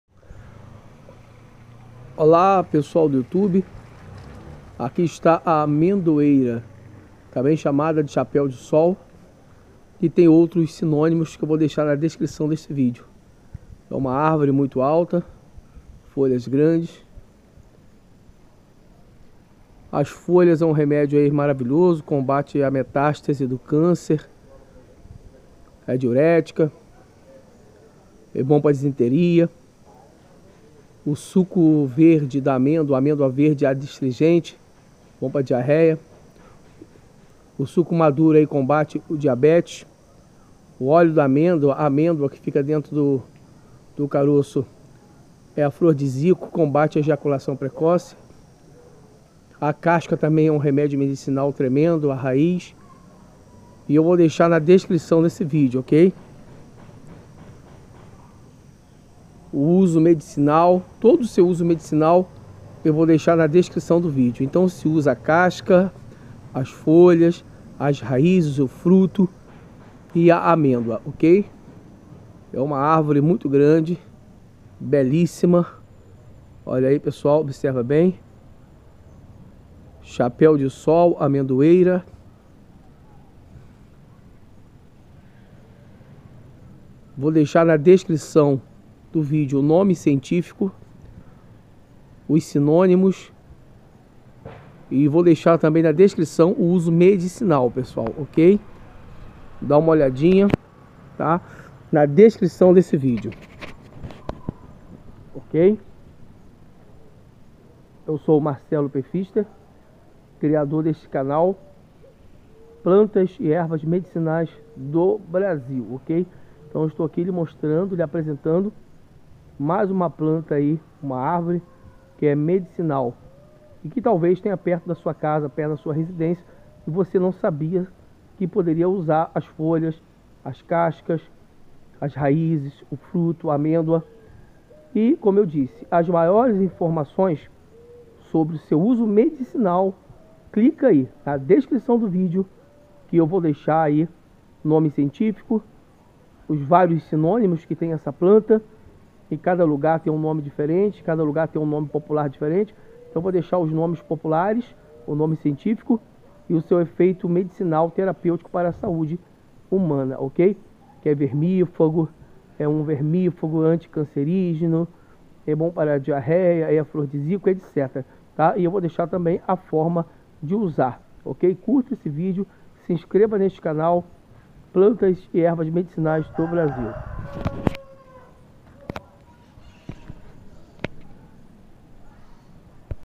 Chapéu - de - sol
Terminalia catappa

Descrição : Planta da família das Combretaceae, também conhecida como sete-copas, amêndoa, amendoeira brava, amendoeira tropical, amendoeira-da-índia, amendoeira-da-praia, amendoeira-do-pará, anoz, árvore-de-anoz, castanhola, guarda-chuva, guarda-sol, noz-da-praia, terminália.

Parte utilizada: Folhas, raízes, frutos, casca, óleo do fruto.

Princípios Ativos: Ácidos elágico, ácidos gálico, ácidos querubínico, taninos .

Propriedades medicinais:Adstringente, anti-hemorroidal, antidisentérica, antiespasmódica, antioxidante, febrífuga, laxante, peitoral, vermífuga.

Indicações: Cólicas, hemorróidas, disenteria, febres gástricas, febres biliares, vermes intestinais, emulsões peitorais.

Modo de usar:

- Folhas: Diurética, cólicas e hemorróidas; evita metástases do câncer etc.

- Raiz e casca: disenteria, febres gástricas e biliares e vermes intestinais;

- Óleo doce da amêndoa: emulsões peitorais;

- Fruto verde: adstringente.
- Fruto maduro: Suco combate diabetes.
Amêndoa : Combate ejaculação precose e ajuda na vida sexual.